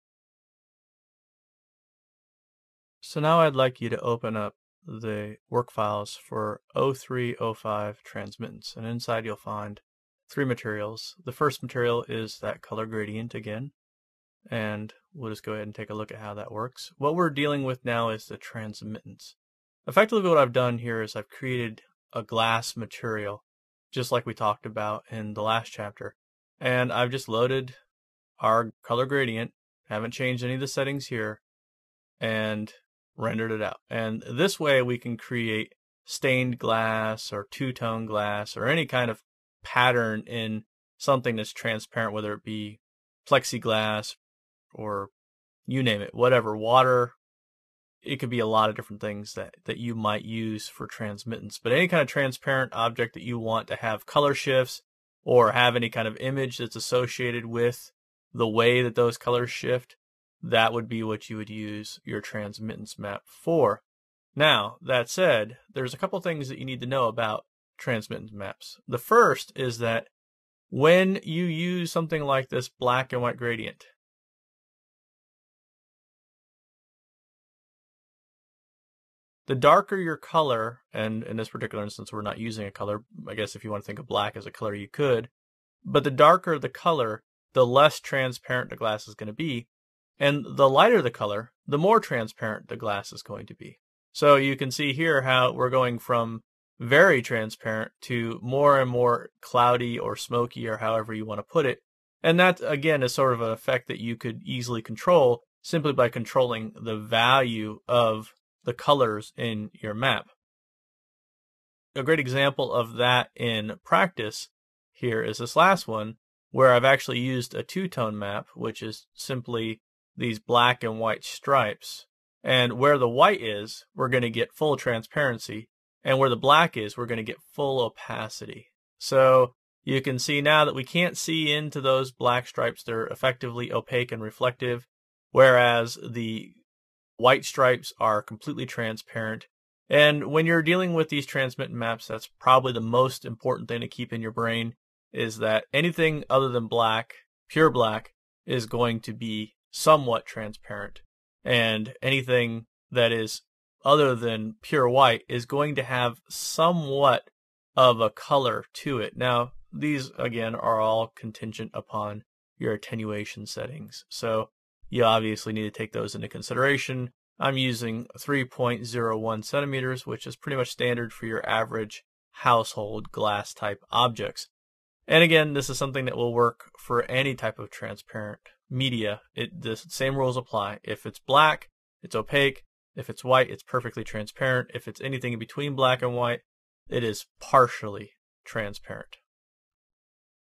Maxwell Render Materials Mapping 5: Transmittance Map
This video is part of Maxwell Render Overview Series created by Jason Maraton.